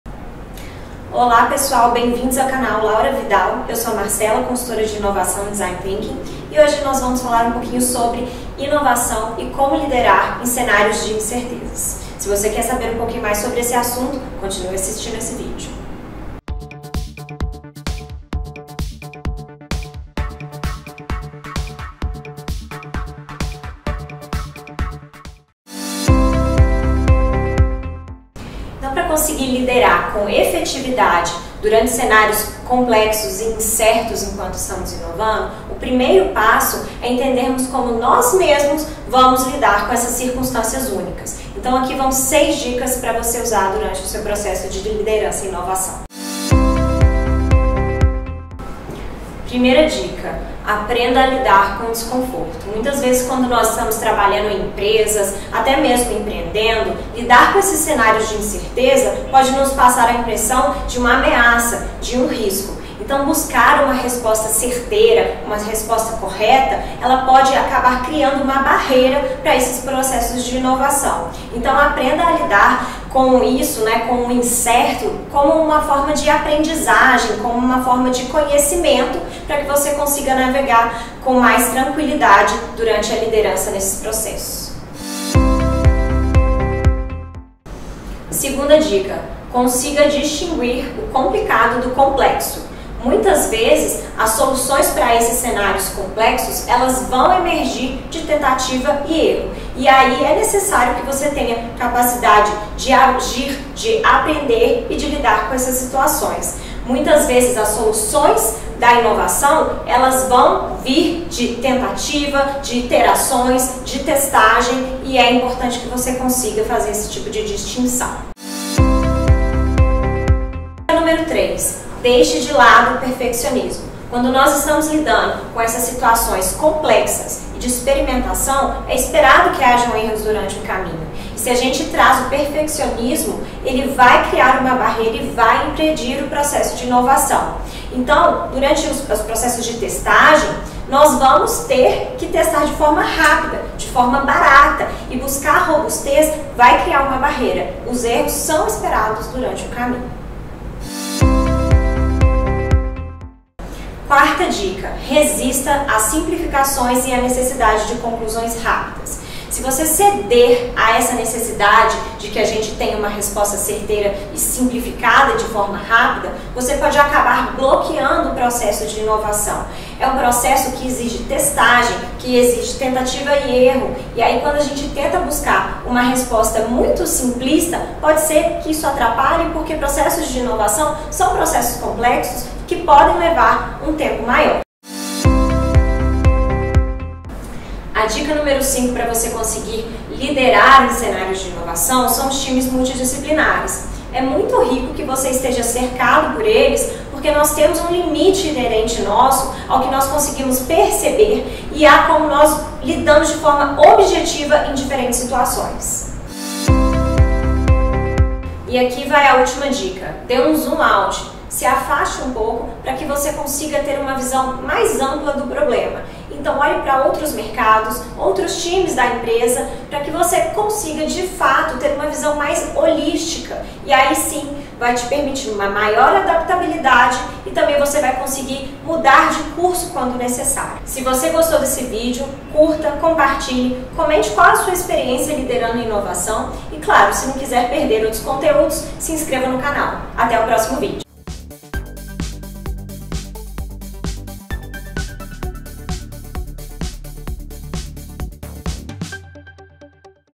Como lidar com as incertezas durante o processo de inovação - Laura Widal
Processos de inovação quando colocados em prática geram valor percebido para pessoas e resultados de negócio. Eles vão requerer algumas características daqueles que estão a sua frente para que consigam liderar da melhor maneira. Saber lidar com as incertezas é uma das necessidades deste perfil e do modelo mental inovador.

Neste vídeo você verá estratégias para melhorar a capacidade de um líder de aprender, crescer e navegar de forma mais eficaz em cenários de inovação, substituindo a mentalidade fixa para a mentalidade de crescimento.